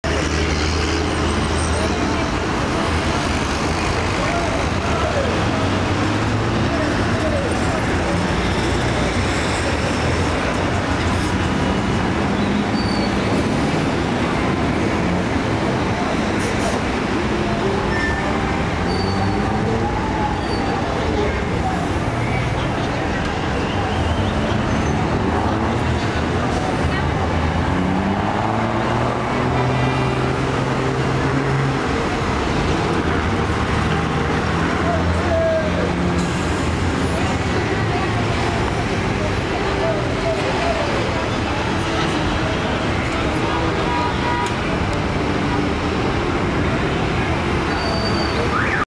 Entrada a Soyapango
Entrada a Soyapango during peak hours. View from Plaza Mundo.  Para los que me escriben que quieren ver mas videos cotidianos, ahi les va este.

Eran las 1400hrs y ya habia bastante trafico.